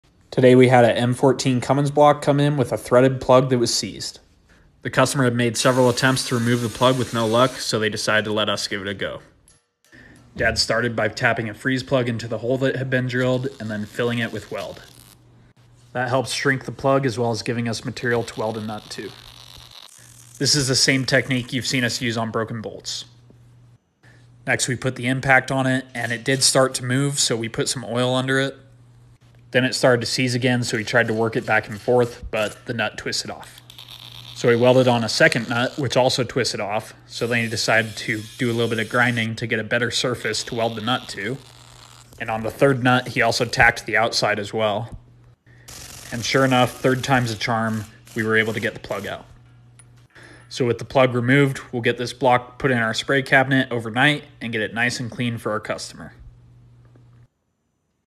N14 Cummins Block Plug Removal - This one fought back!
Sometimes they fight back a bit! But the welder never fails (even in the hands of a machinist)